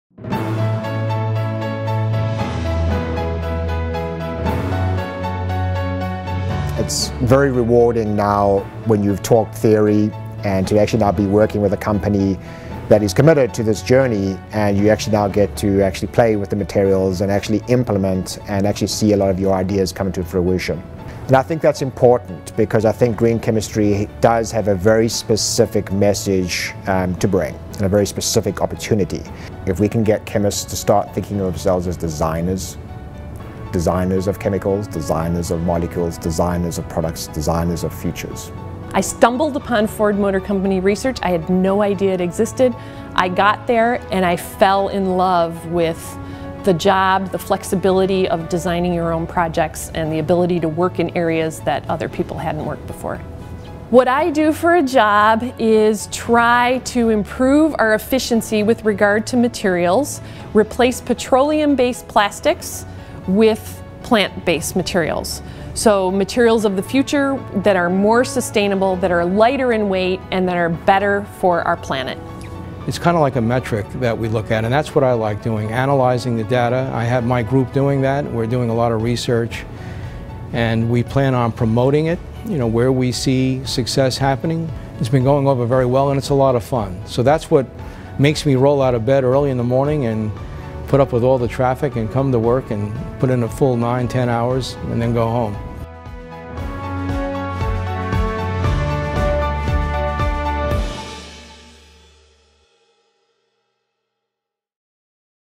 What Makes You Passionate About Green Chemistry?
Save the Date for the 2016 Green Chemistry and Engineering Conference, November 17, 2016!
Green Chemistry is changing the way we design products, reducing our impact on the environment and promises to deliver a more sustainable future. At the 2015 GreenUp Conference, we asked three Green Chemistry leaders to tell us what makes them passionate about their field. Hear what these industry leaders have to say and visit Michigan.gov/greenchemistry to learn more!

who is featured,
Dr. Deborah Mielewski, Senior Technical Leader of Sustainable Materials and Plastics Research, Ford Motor Company
Stephen C. DeVito, PhD, R.Ph., U.S. Environmental Protection Agency
Dr. Clinton Boyd, Steelcase